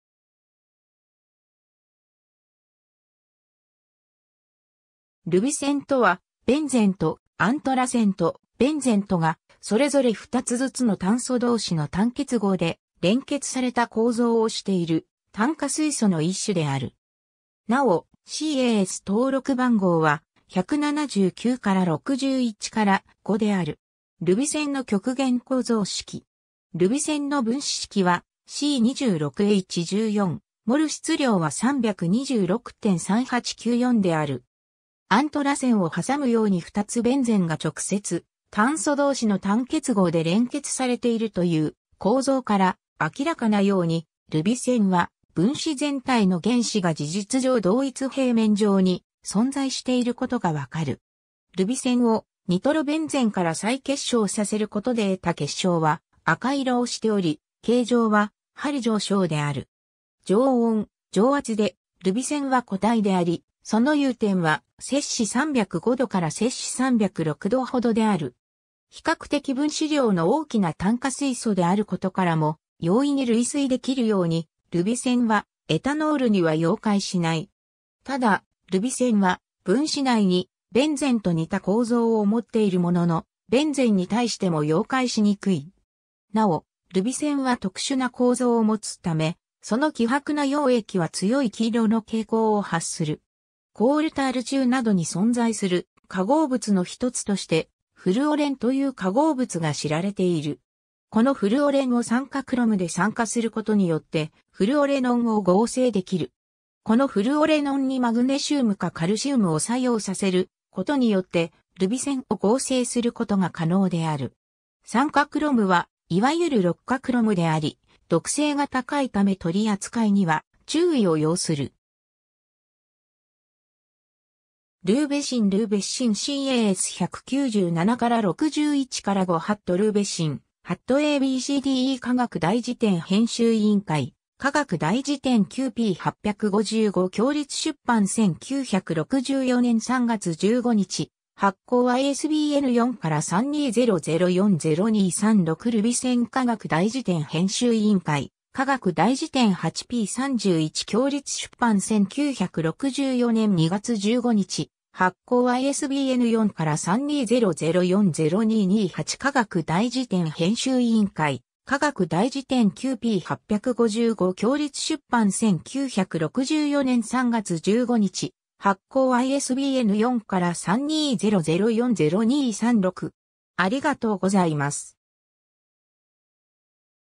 ルビセン
ルビセン（Rubicene）とは、ベンゼンとアントラセンとベンゼンとが、それぞれ2つずつの炭素同士の単結合で連結された構造をしている、炭化水素の1種である。
なお、CAS登録番号は、179-61-5である。
 ルビセンの極限構造式。
 ルビセンの分子式はC26H14、モル質量は326.3894 (g/mol)である 。
 アントラセンを挟むように2つベンゼンが直接炭素同士の単結合で連結されているという構造から明らかなように、ルビセンは分子全体の原子が事実上同一平面上に存在していることが判る 。
 ルビセンをニトロベンゼンから再結晶させることで得た結晶は赤色をしており、形状は針状晶である 。
 常温・常圧でルビセンは固体であり、その融点は305 ℃ から306 ℃ ほどである。
比較的分子量の大きな炭化水素であることからも容易に類推できるように、ルビセンはエタノールには溶解しない 。
 ただ、ルビセンは分子内にベンゼンと似た構造を持っているものの、ベンゼンに対しても溶解しにくい 。
 なお、ルビセンは特殊な構造を持つため、その希薄な溶液は強い黄色の蛍光を発する 。
 コールタール中などに存在する化合物の1つとしてフルオレンという化合物が知られている。
このフルオレンを酸化クロム(VI)で酸化することによって、フルオレノンを合成できる 。
 このフルオレノンにマグネシウムかカルシウムを作用させることによって、ルビセンを合成することが可能である 。
 ^ 酸化クロム(VI)は、いわゆる六価クロムであり、毒性が高いため取り扱いには注意を要する。
 ^ Rubicene- ^ Rubicene - CAS # 197-61-5 ^ Rubicene (3D Conformer) ^ a b c d e 化学大辞典編集委員会 『化学大辞典 （縮刷版） 9』 p.855 （右上） 共立出版 1964年3月15日発行 ISBN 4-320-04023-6 ^ ルビセン ^ 化学大辞典編集委員会 『化学大辞典 （縮刷版） 8』 p.31 （左上） 共立出版 1964年2月15日発行 ISBN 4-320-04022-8 ^ 化学大辞典編集委員会 『化学大辞典 （縮刷版） 9』 p.855 （右上） 共立出版 1964年3月15日発行 ISBN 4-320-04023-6